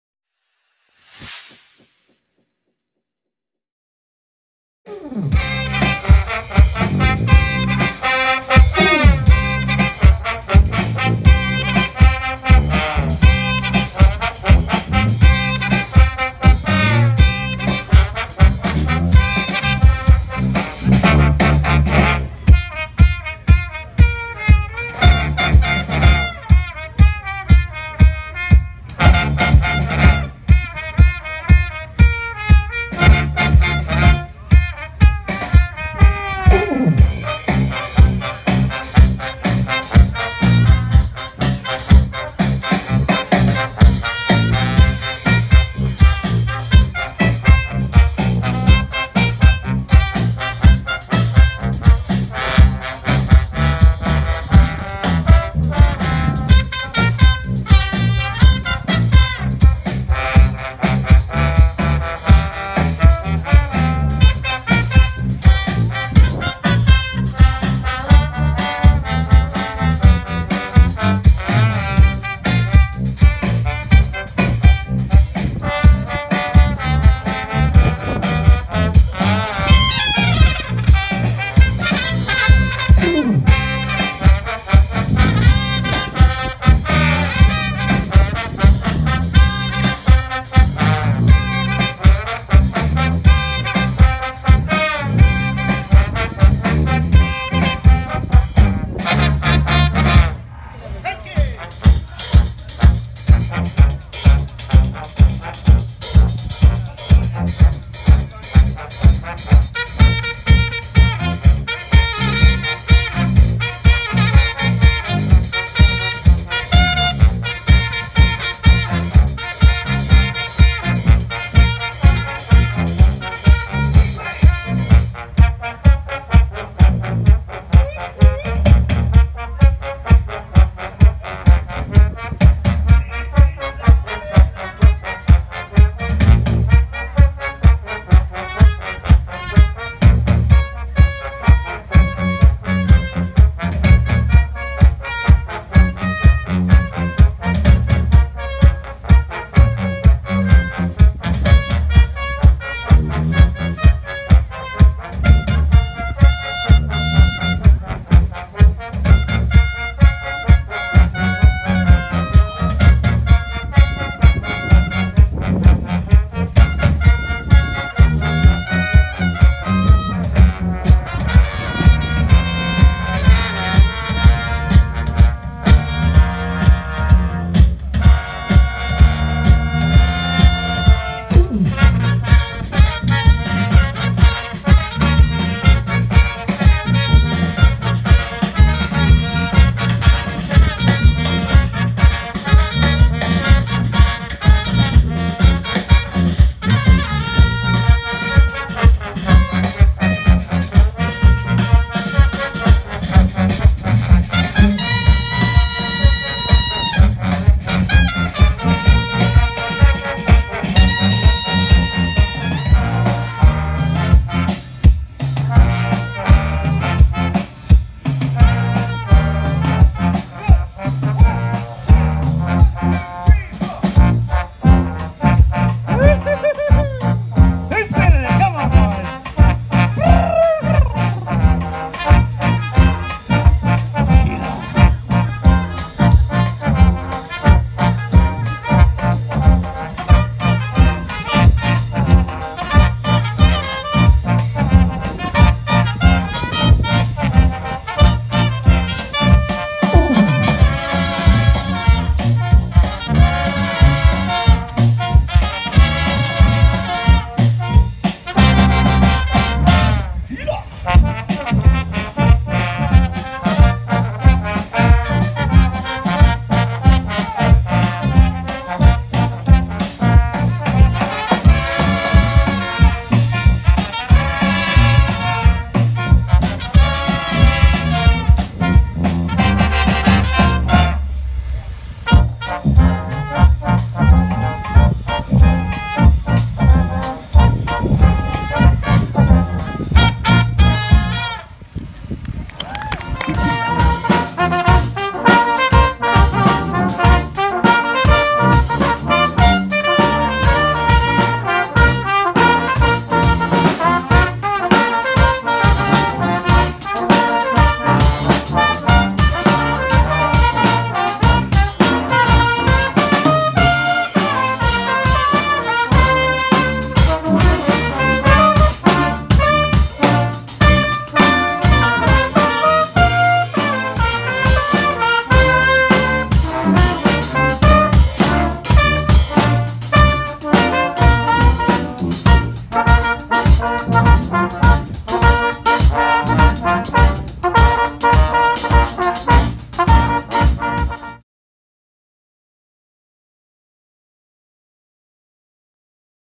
VIBE CITY STREET BRASS @ Rainbow Summer Solstice Festival 16/6/18
Vibe City performing at the Rainbow Summer Solstice Festival June 16th 2018 great day !